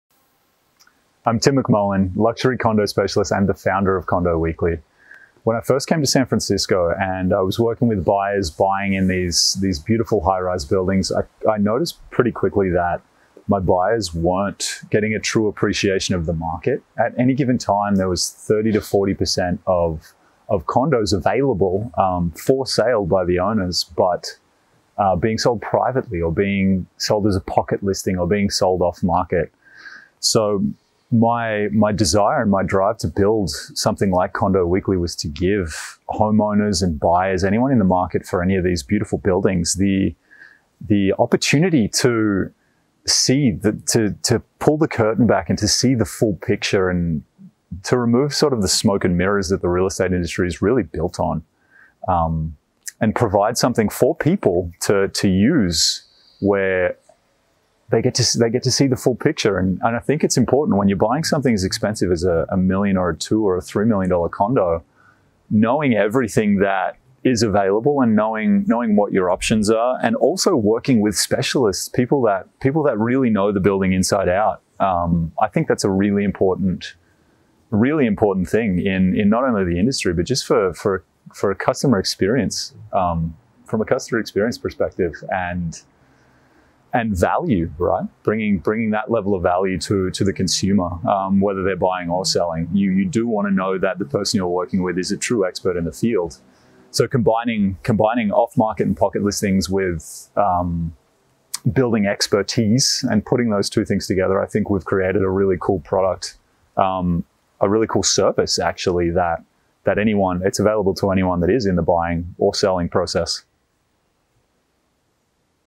Creating Efficiency In Condominium Sales
There should be an easier way to buy & sell homes in highrise buildings..